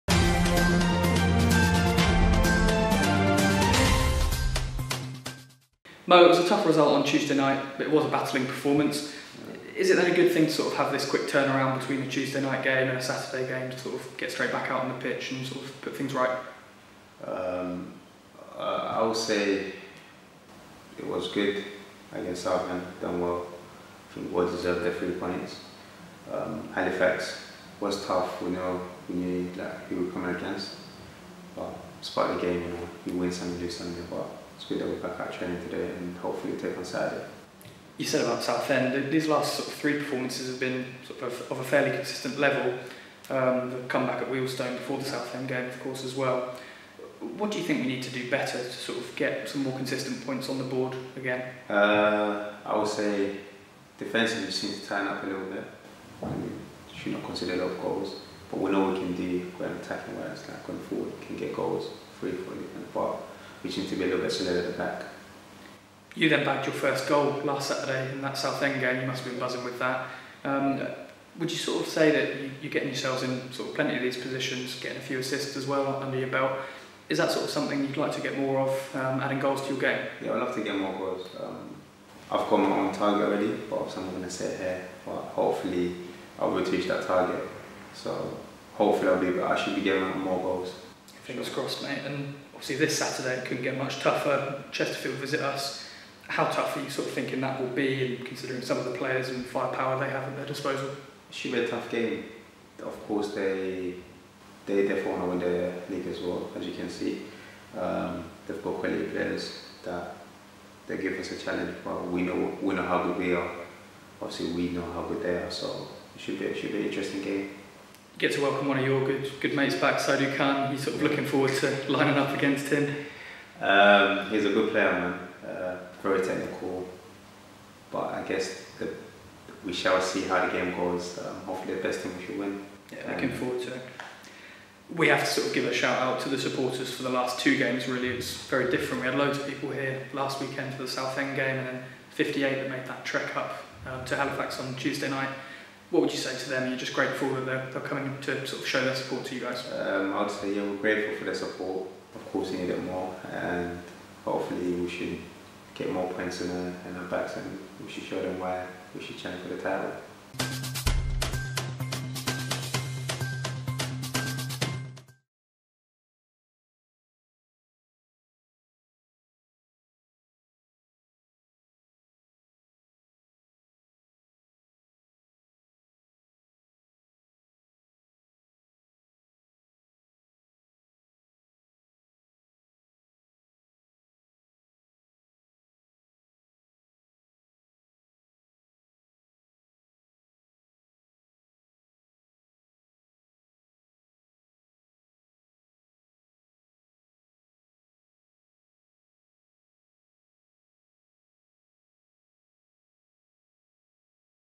Mo Sagaf Looks Ahead To Chesterfield (H)
DaggersTV heard from Mo Sagaf after training this afternoon, to reflect on recent games and to look ahead to this weekend's game against Chesterfield.